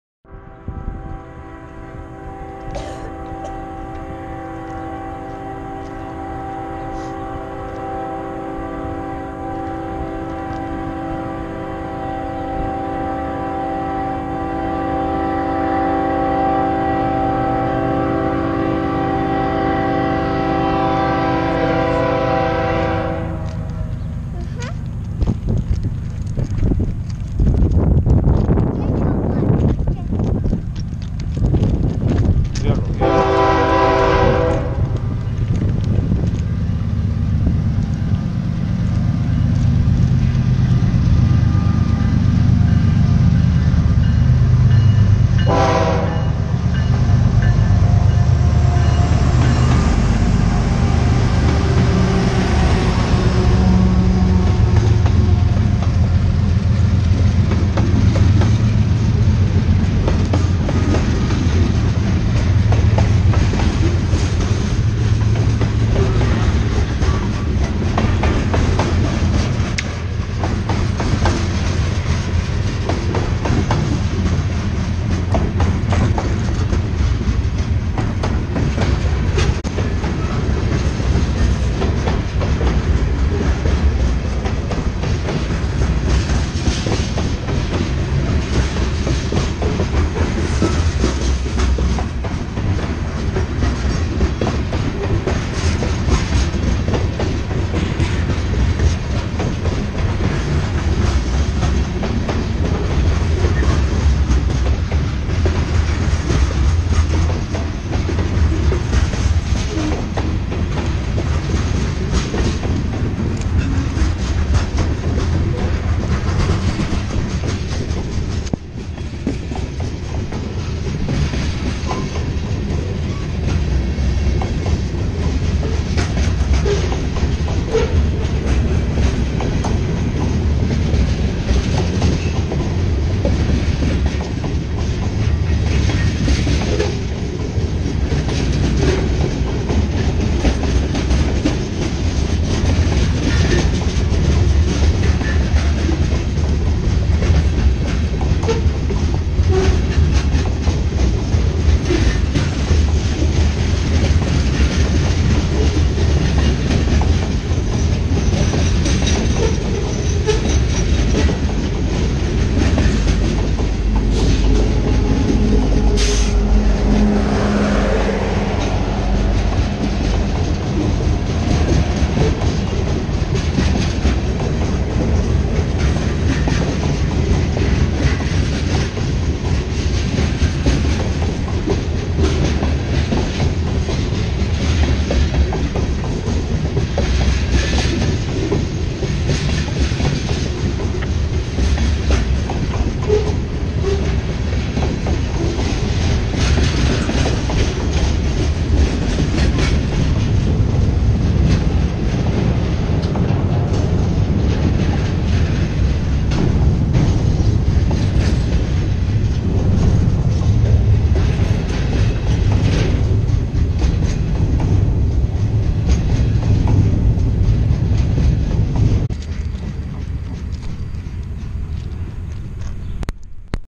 Tren entrando a Guadalajara en Las Juntas Jalisco.
Espero sus comentarios constructivos.